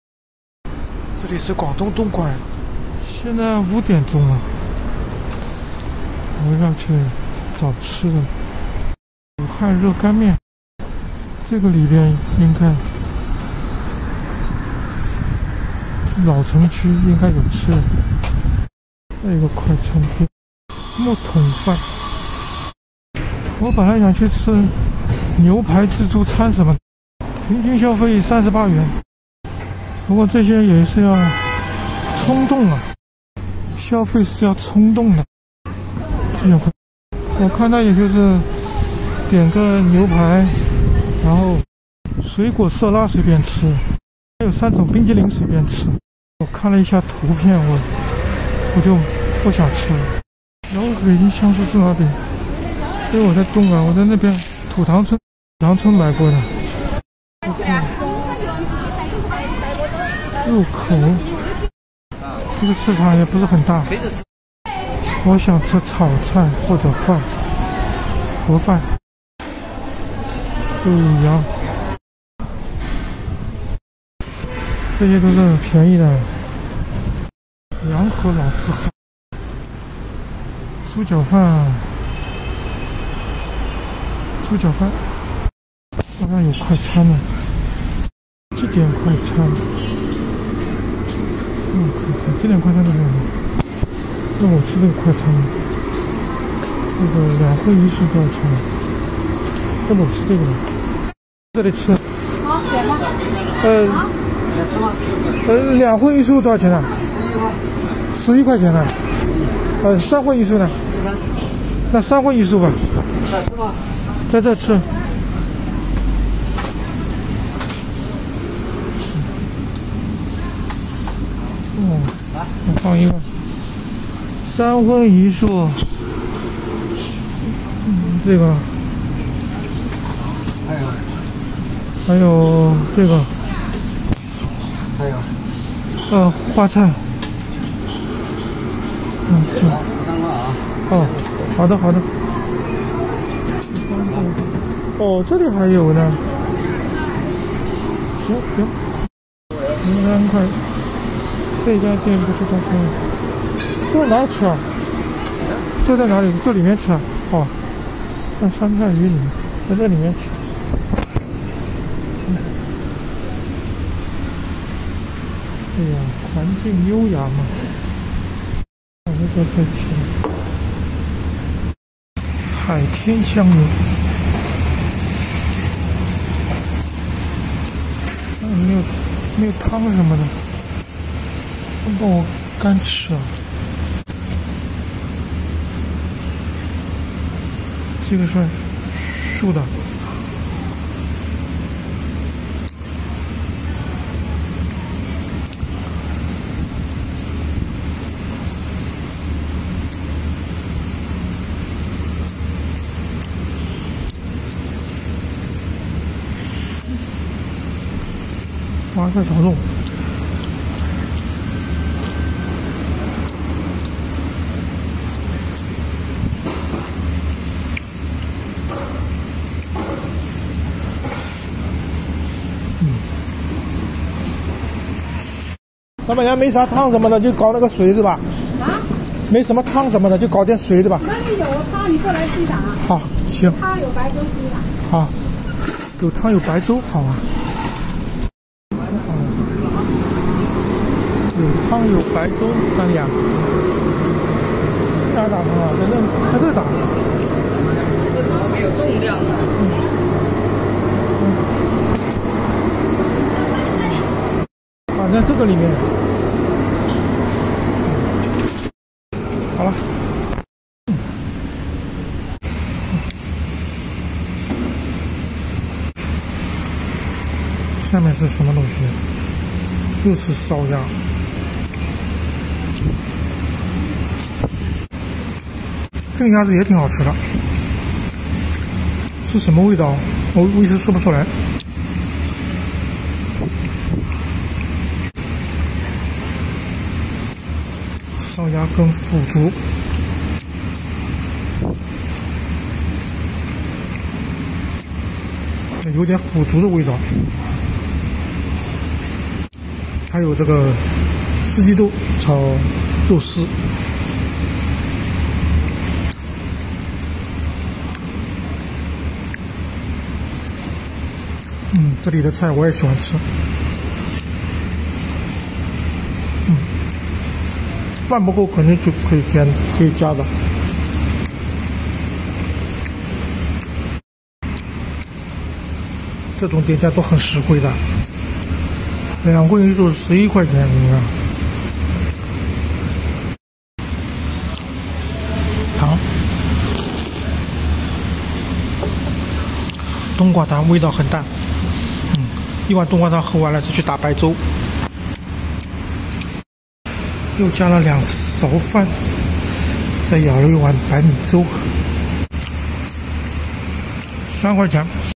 旅行廣東東莞吃壹份三葷壹素13塊人民幣的快餐，感覺挺實惠的
旅行广东东莞吃一份三荤一素13块人民币的快餐，感觉挺实惠的